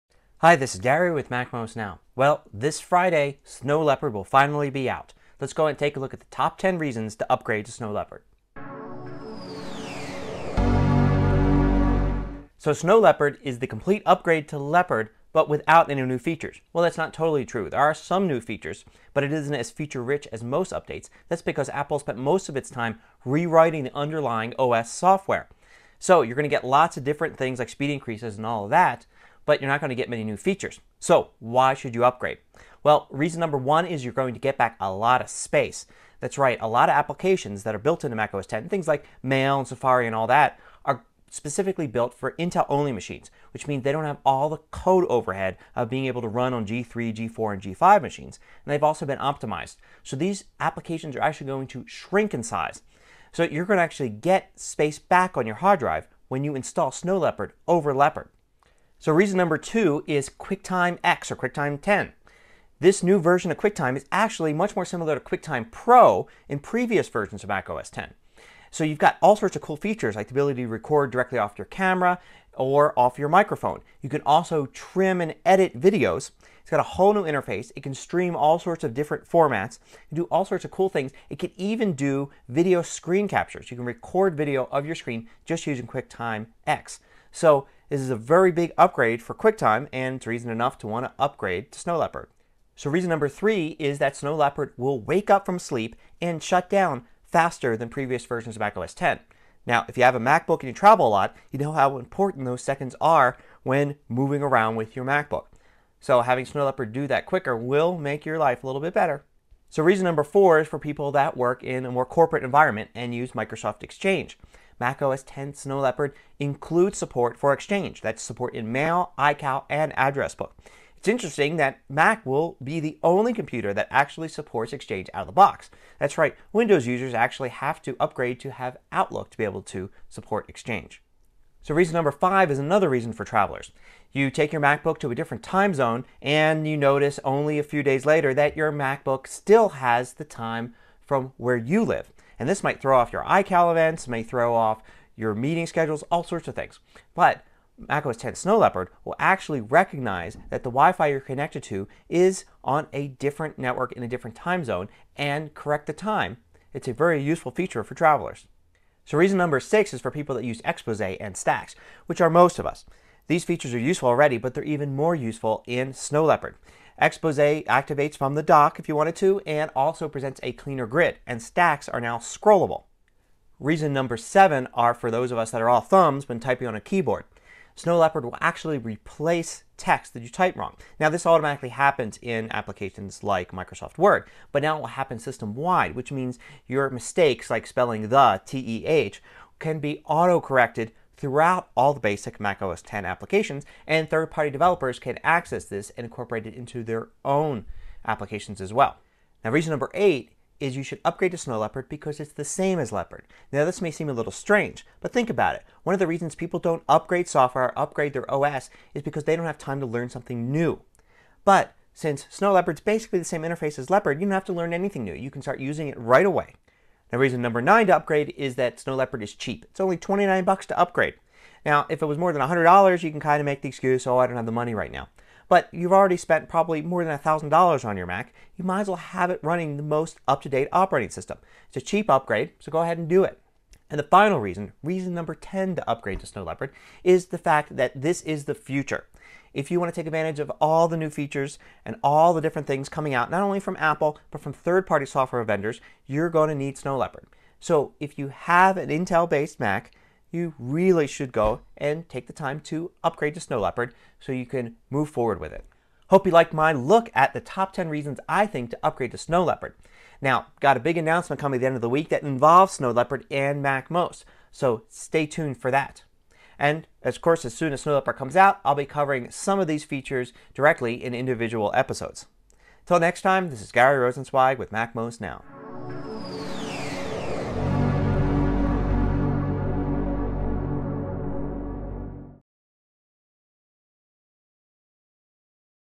Top 10 Reasons to Upgrade to Snow Leopard (MacMost Now 283)
Gary Rosenzweig runs down his top 10 reasons to get Snow Leopard, Apple's new OS for Intel-based Macs.